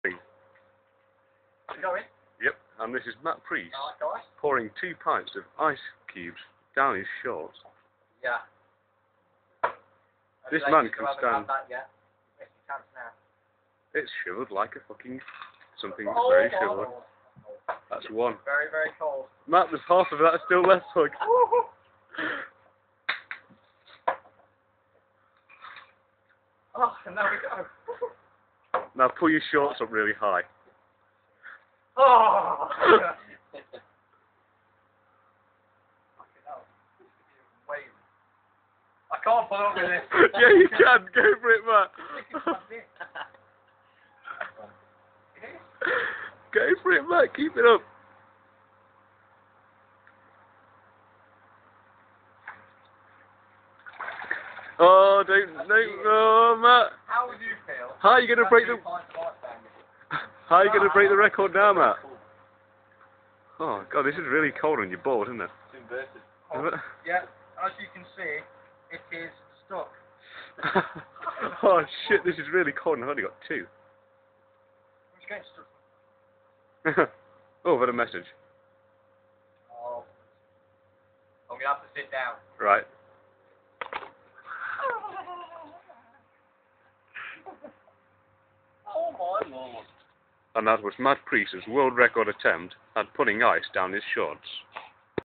Ice down shorts world record attempt
Well its not really a world record attempt, its just funny, and painfull.